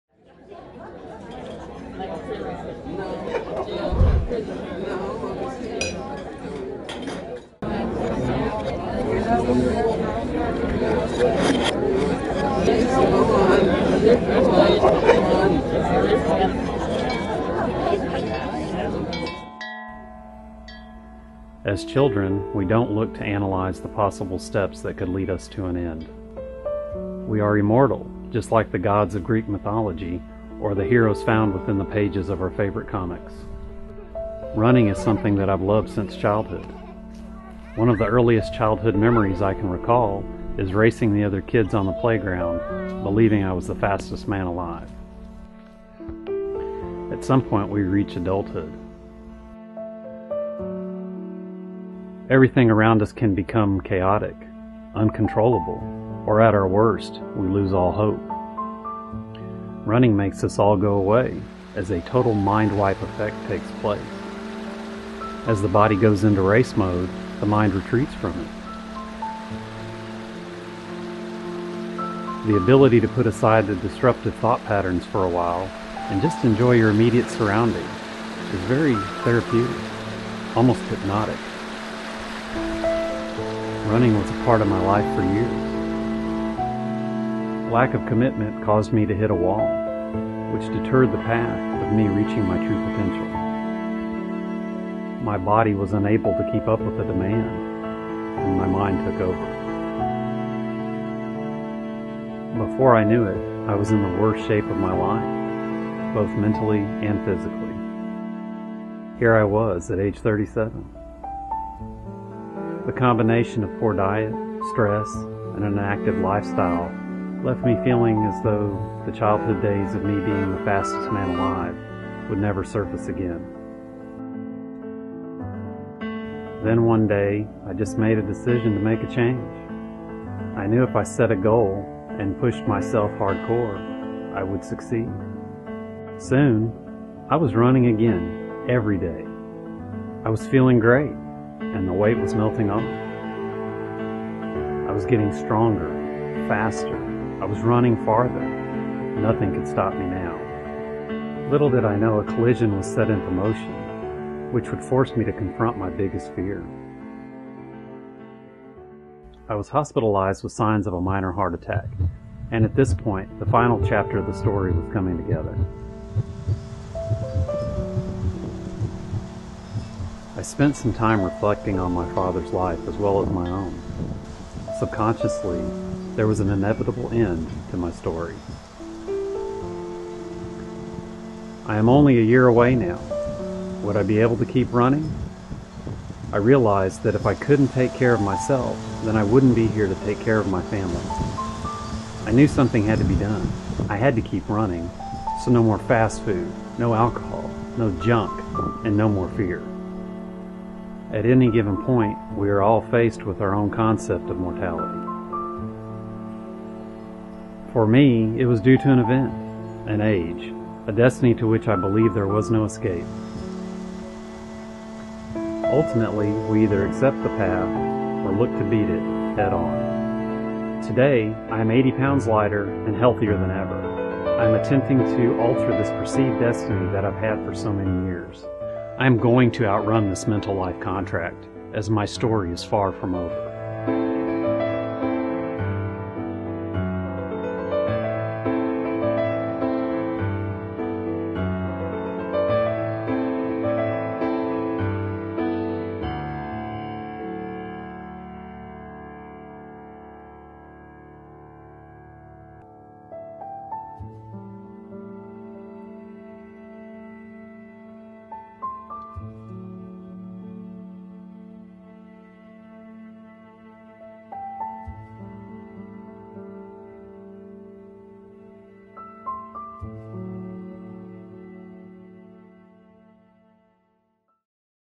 Chasing 40 - 5D Mark II
Follow Jimmie's story as he is confronted with life's biggest obstacles and how he is managing to overcome them. Experience this personal testimonial about life, death, and hope.